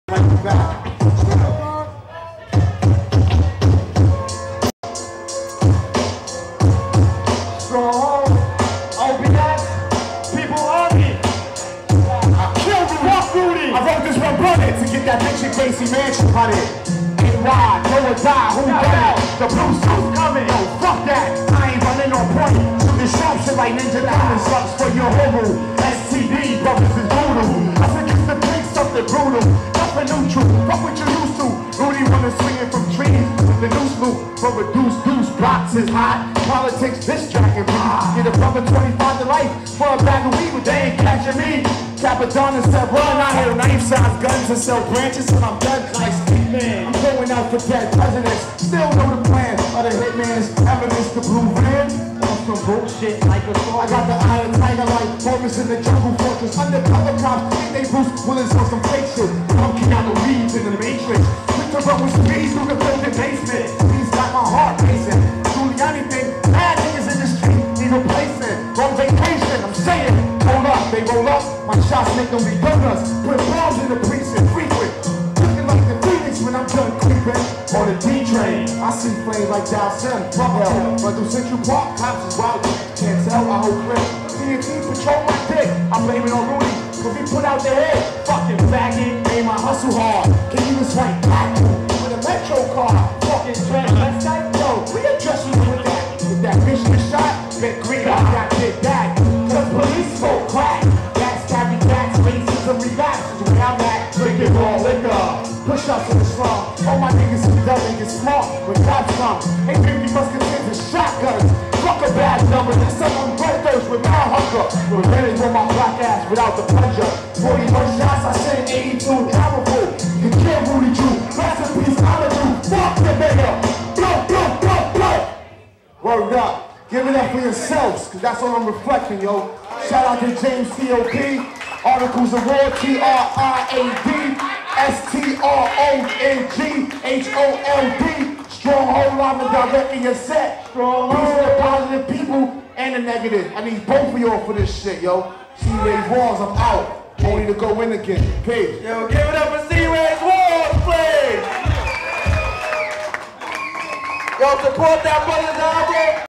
C RAYZ WALZ @ Culture Club produced by SidLocks
Another SidLocks production... C Rayz Walz. This Artist is on my top 10 tru-skool Mcees. It's always a pleasure to document this energetic and humorous MCee. Produced by SidLocks for Multihop/Enternational Nutz Media